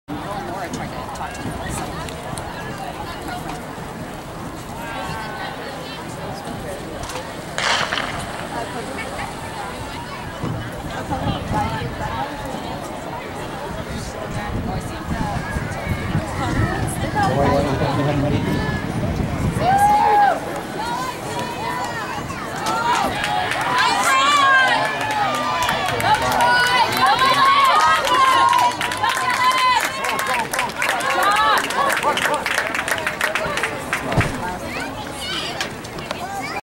OpenB-1 200m vs Marina & NH 4-3-14 - Los Alamitos Boys
Los Alamitos HS Open Boys Heat 1 200m
vs Marina HS and Newport Harbor HS 4-3-14